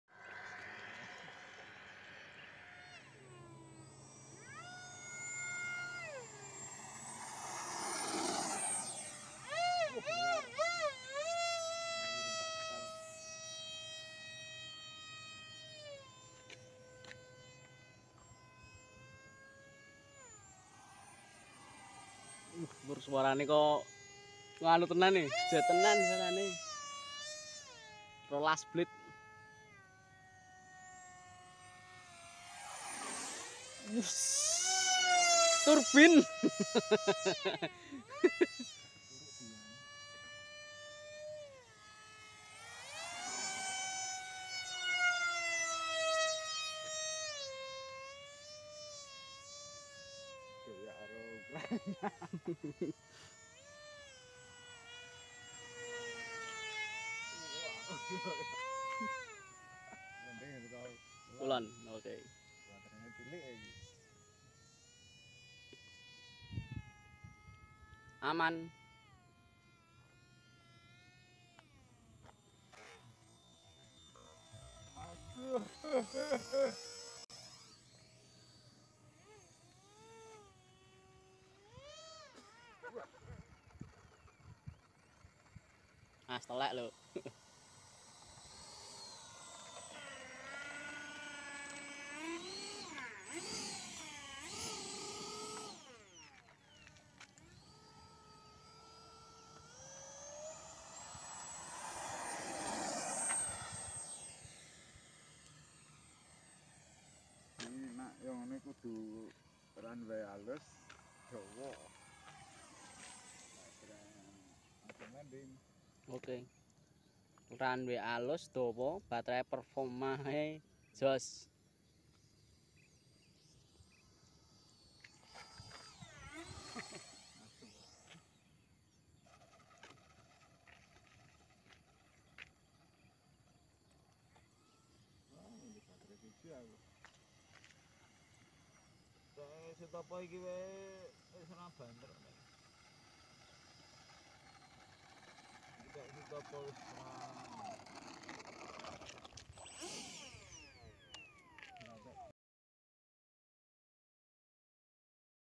Maiden Viper Jet with EDF
VIPER JET STYROFOAM HOMEMADE

Spesifikasi :
WS 800 mm
EDF 5,5 12 blade
Esc 80 A
Battery Lipo 3S 1500mAh
Servo 5 x 9 gram
TX/RX : Jumper T-Lite/IA6b
Pilot : Badari Erzet